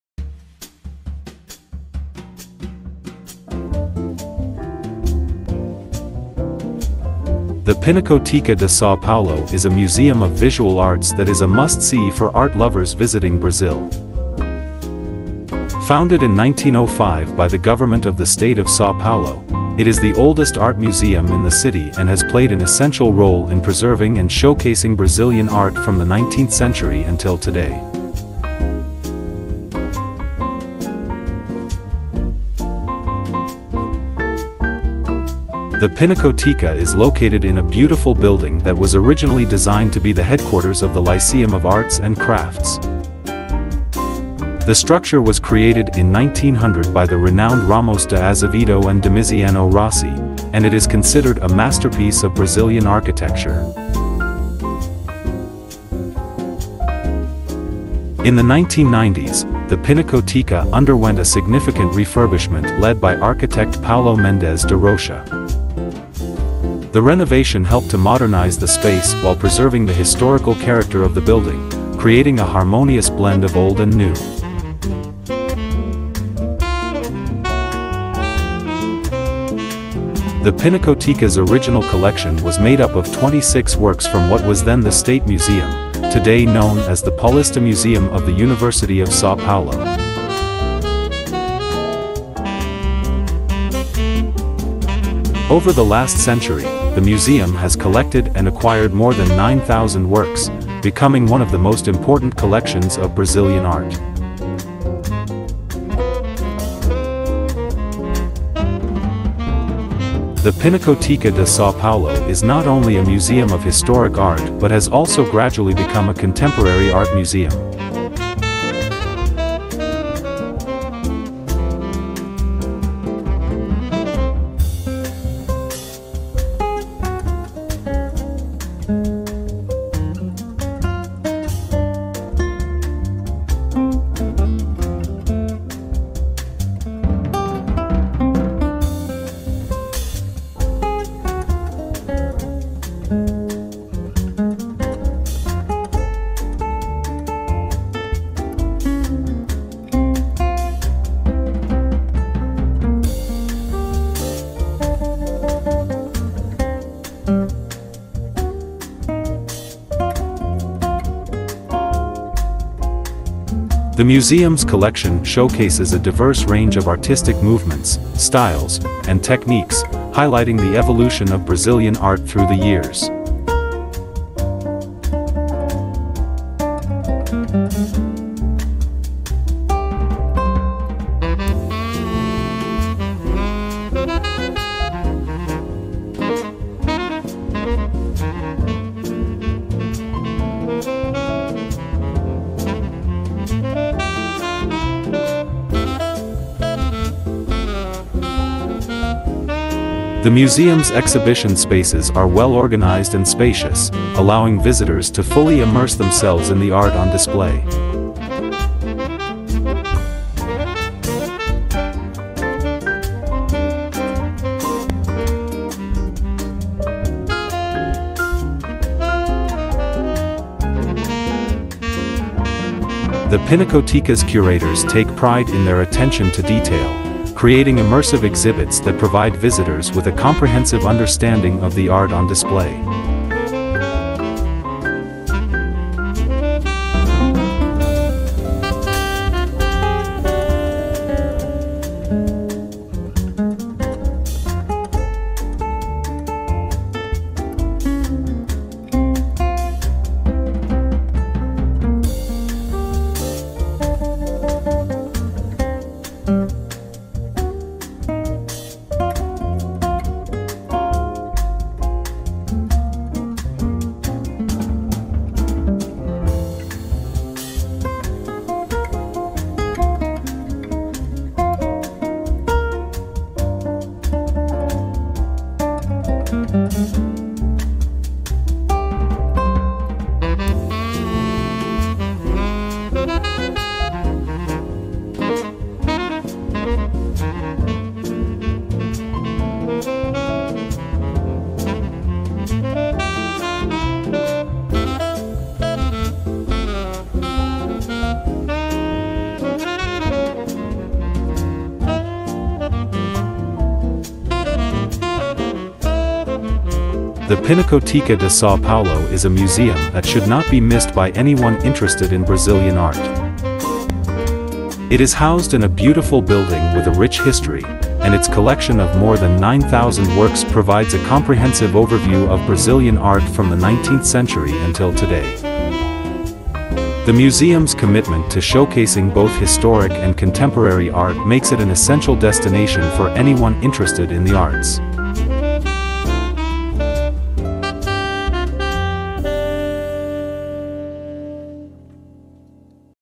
Brazil: Pinacoteca de Sao Paulo, Brazil
The Pinacoteca de Sao Paulo is a museum of visual arts that is a must-see for art lovers visiting Brazil.  Founded in 1905 by the Government of the State of Sao Paulo, it is the oldest art museum in the city and has played an essential role in preserving and showcasing Brazilian art from the 19th century until today. The Pinacoteca is located in a beautiful building that was originally designed to be the headquarters of the Lyceum of Arts and Crafts.

The structure was created in 1900 by the renowned Ramos de Azevedo and Domiziano Rossi, and it is considered a masterpiece of Brazilian architecture. In the 1990s, the Pinacoteca underwent a significant refurbishment led by architect Paulo Mendes da Rocha.  The renovation helped to modernize the space while preserving the historical character of the building, creating a harmonious blend of old and new.

The Pinacoteca's original collection was made up of 26 works from what was then the State Museum, today known as the Paulista Museum of the University of Sao Paulo.  Over the last century, the museum has collected and acquired more than 9,000 works, becoming one of the most important collections of Brazilian art.
The Pinacoteca de Sao Paulo is not only a museum of historic art but has also gradually become a contemporary art museum.

The museum's collection showcases a diverse range of artistic movements, styles, and techniques, highlighting the evolution of Brazilian art through the years. The museum's exhibition spaces are well-organized and spacious, allowing visitors to fully immerse themselves in the art on display.

The Pinacoteca's curators take pride in their attention to detail, creating immersive exhibits that provide visitors with a comprehensive understanding of the art on display.